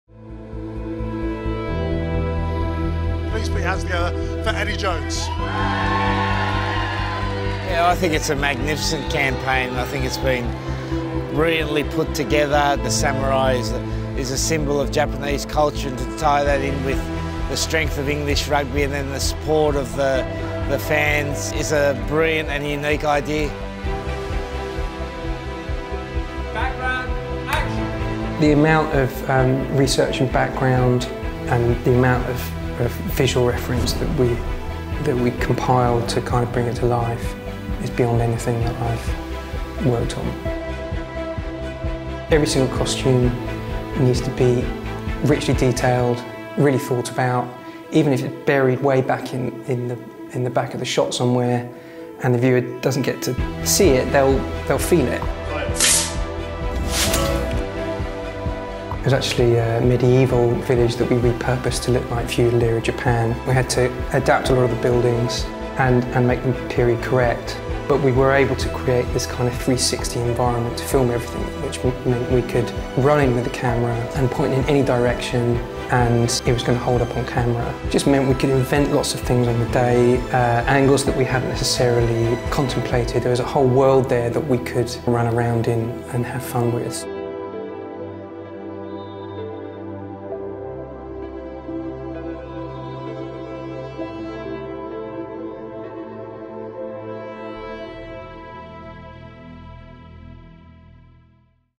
Be their Armour campaign - Behind the scenes
As lead sponsor of the England Rugby team since 1995, we couldn't be prouder of the team's journey in Japan and how fans responded to our Be their Armour campaign, showing incredible support for the team this autumn. See how we made it here.